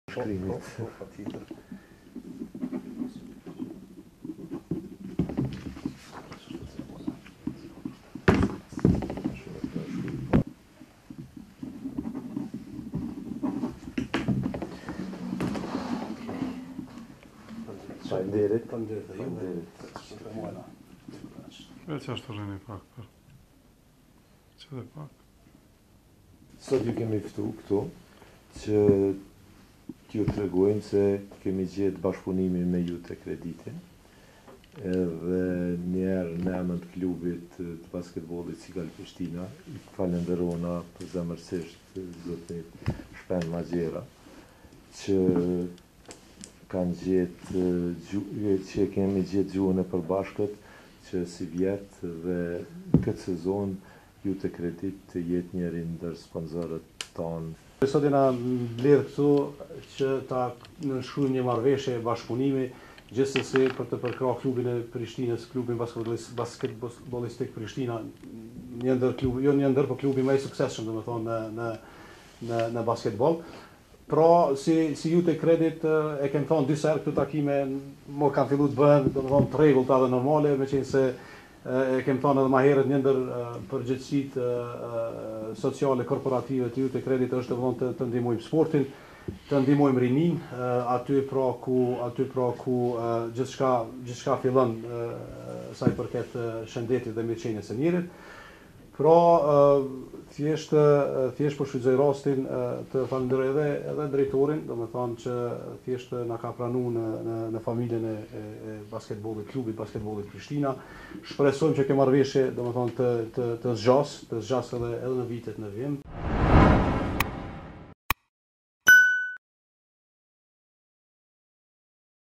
Ky është sponsori i ri i KB Prishtina
Klubi basketbollistik, Sigal Prishtina, ka arritur marrëveshje me IUTE Credit.